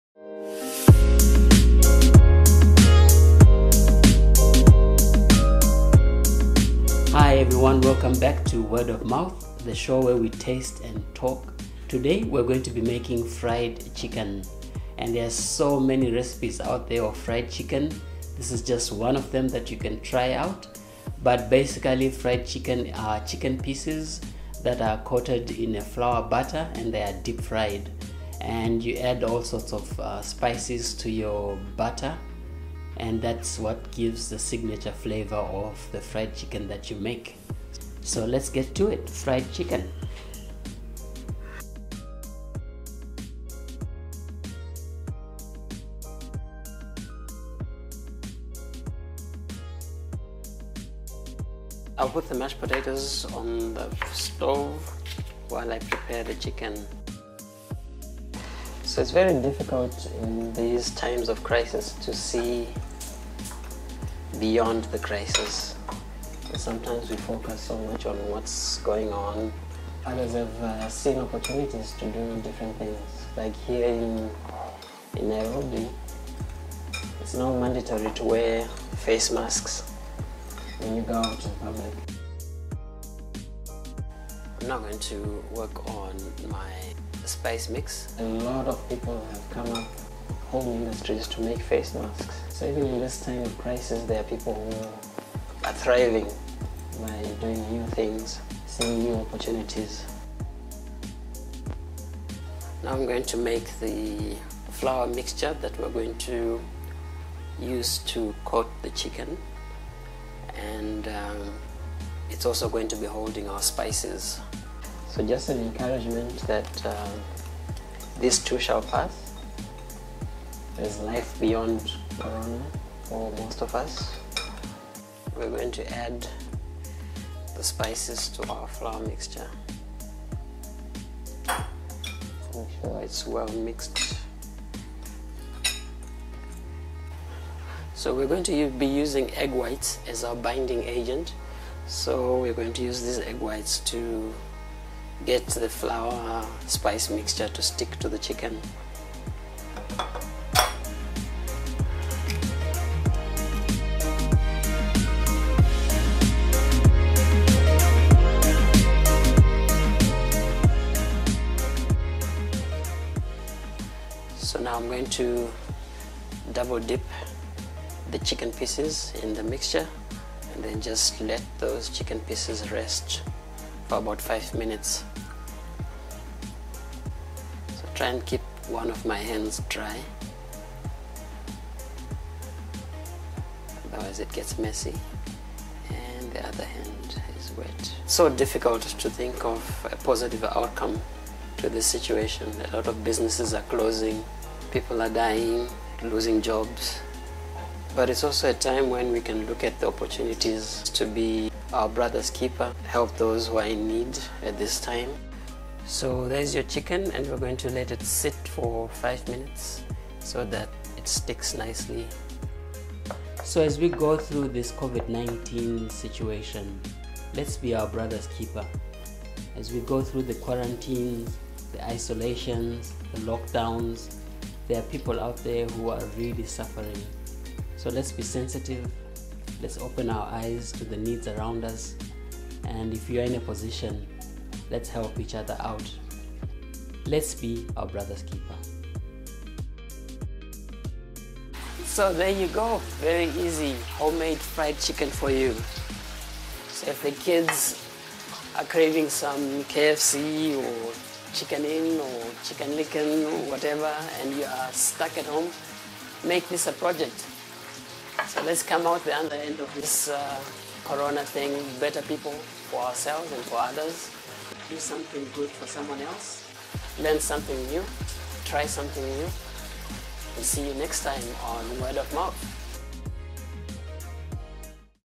WoM - Episode 8 - Fried Chicken
Fried chicken is a favorite of many. You can make yourself this treat at home easily. You can even come up with your own version by just varying the herbs and spices to your taste. Fried chicken pairs well with fries or mashed potatoes.